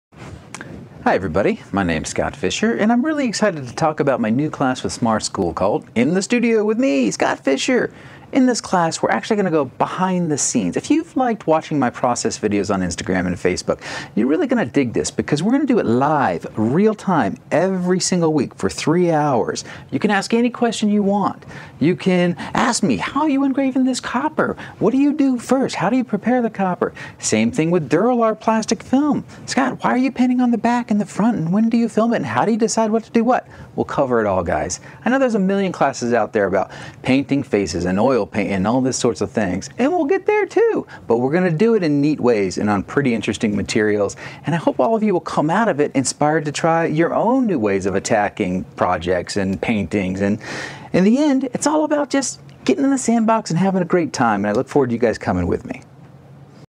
SmArt School: In the Studio w/ Scott M Fischer
IN THE STUDIO: Boundary pushing techniques using traditional and digital materials. A 14 week class hosted by SmArt School.